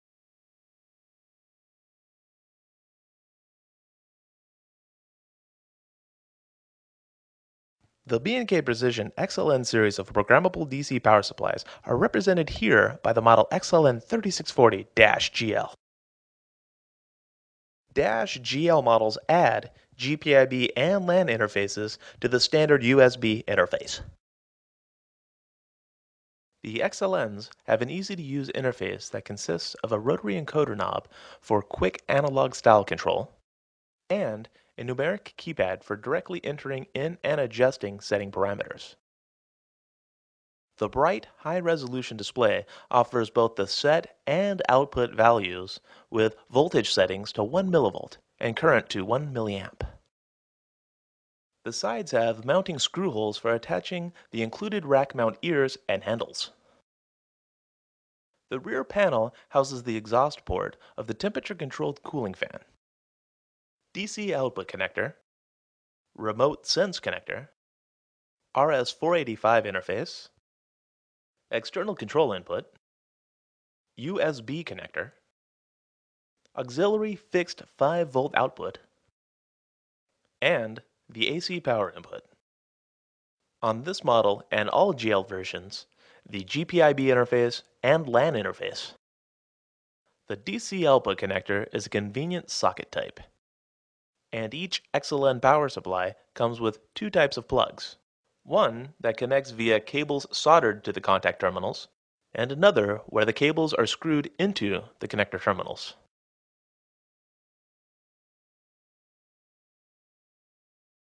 BK Precision XLN Series Programmable DC Power Supplies XLN3640
The B&K Precision XLN series are compact, programmable, single-output DC power supplies, suitable for a wide range of applications. Comparable supplies from other manufacturers primarily address the ATE market, while the XLN series are designed for both bench top users and system integrators.

For bench top applications, these power supplies offer built-in voltage and current meters displaying setting and output values concurrently, as well as an intuitive user interface with full keypad and rotary knob. Free application software is available to provide remote control capabilities without the need for any computer programming. Standard USB & RS485 and optional GPIB & LAN interfaces combined with fast average command processing times of less than 50 ms make the XLN series ideal for ATE applications. A complete set of LabVIEW drivers are also available to reduce programming time and increase productivity.